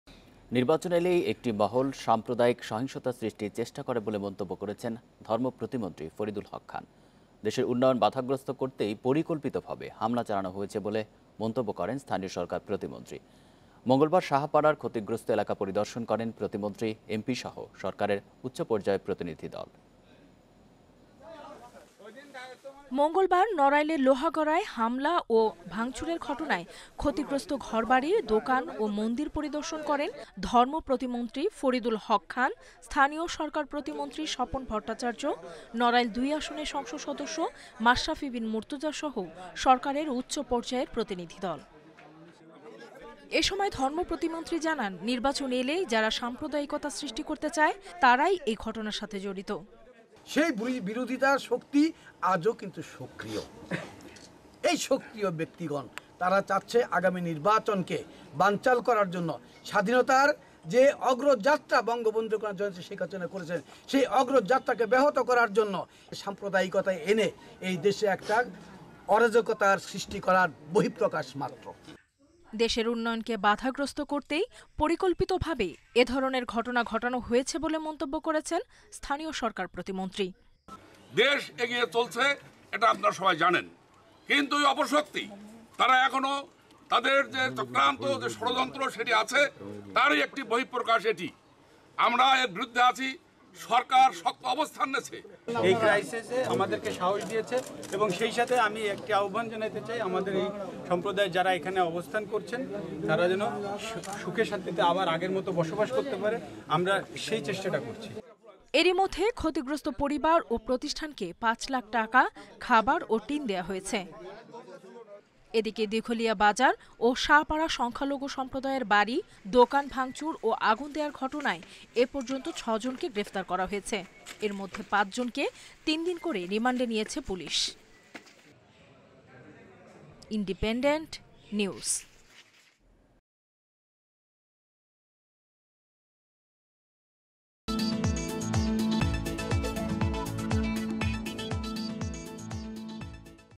'দেশের উন্নয়ন বাধাগ্রস্ত করতেই পরিকল্পিতভাবে এ ঘটনা ঘটানো হয়েছে ' || FLW Minister Visit
নির্বাচন এলেই একটি মহল সাম্প্রদায়িক সহিংসতা সৃষ্টির চেষ্টা করে বলে মন্তব্য করেছেন ধর্ম প্রতিমন্ত্রী ফরিদুল হক খান। দেশের উন্নয়ন বাধাগ্রস্ত করতেই পরিকল্পিতভাবে হামলা চালানো হয়েছে বলে মন্তব্য করেন--স্থানীয় সরকার প্রতিমন্ত্রী। মঙ্গলবার সাহাপাড়ার ক্ষতিগ্রস্ত এলাকা পরিদর্শন করেন প্রতিমন্ত্রী-এমপিসহ সরকারের উচ্চ পর্যায়ের প্রতিনিধি দল।

মঙ্গলবার নড়াইলের লোহাগড়ায় হামলা ও ভাংচুরের ঘটনায় ক্ষতিগ্রস্ত ঘরবাড়ি, দোকান ও মন্দির পরিদর্শন করেন--ধর্ম প্রতিমন্ত্রী ফরিদুল হক খান, স্থানীয় সরকার প্রতিমন্ত্রী স্বপন ভট্টাচায্য, নড়াইল-২ আসনের সংসদ সদস্য মাশরাফি বিন মুর্তজাসহ সরকারের উচ্চ পর্যায়ের প্রতিনিধি দল।